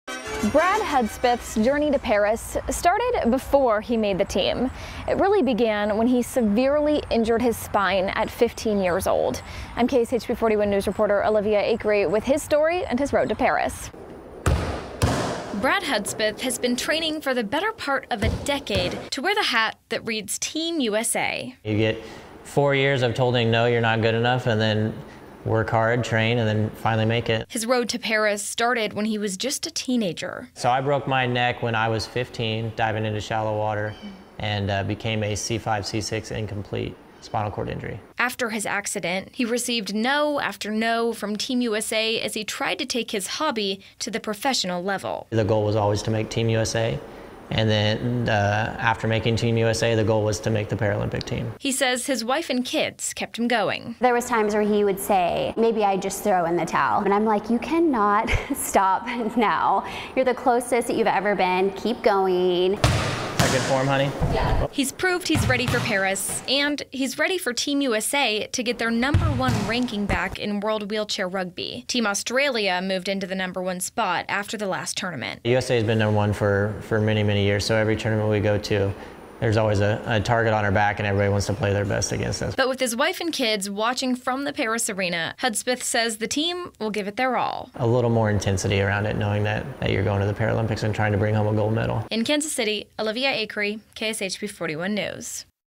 Kansas man to represent Team USA in wheelchair rugby at 2024 Paris Paralympics
Brad Hudspeth is getting ready for Paris, where he’ll represent Team USA in wheelchair rugby.